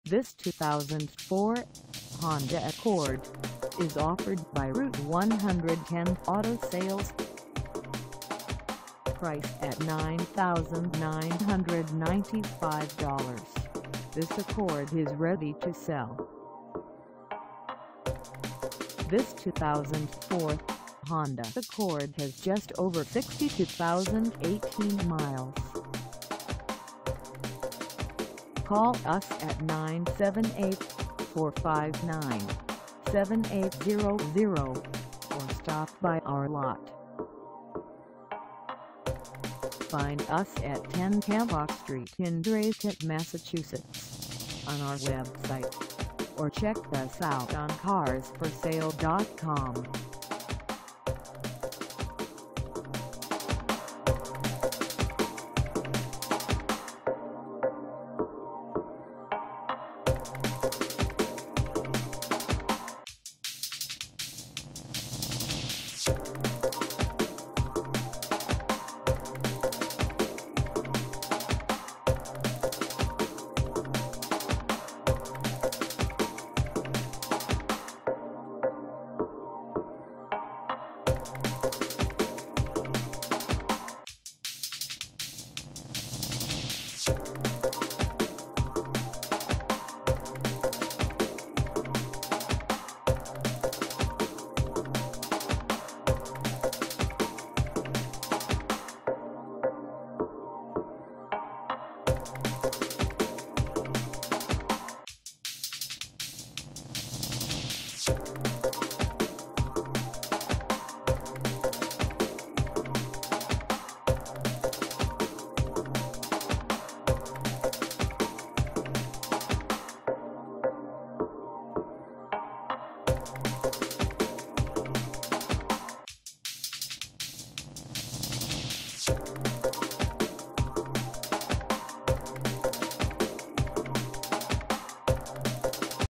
2004 Honda Accord EX sedan AT - for sale in Dracut, MA 01826
Route 110 Auto Sales
10 Hemlock St
in Dracut, MA 01826

Come test drive this 2004 Honda Accord EX sedan AT for sale in Dracut, MA.

The electronic components on this vehicle are in working order.  There are no known defects in this vehicle.  There is not a door ding on this vehicle.  The engine is mechanically sound and can be driven anywhere with confidence.  The exterior finish on this automobile is in excellent condition.  The previous owner was a non-smoker.  The interior is in extra clean condition.  The mileage represented on this vehicle is accurate.  The transmission shifts like it was new.  This is a one-owner vehicle.  This vehicles tires are only slightly worn with 75% of tread life left.  WARRANTY IS AVAILABLE.